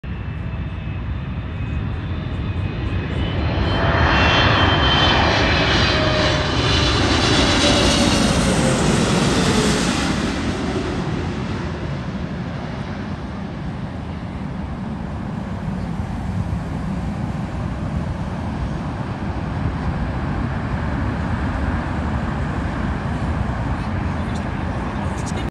China Eastern Airlines Boeing 777-300 (and film crew!) – LAX March 29, 2024
Experience the thrill of takeoffs and landings from one of the best vantage points in the world.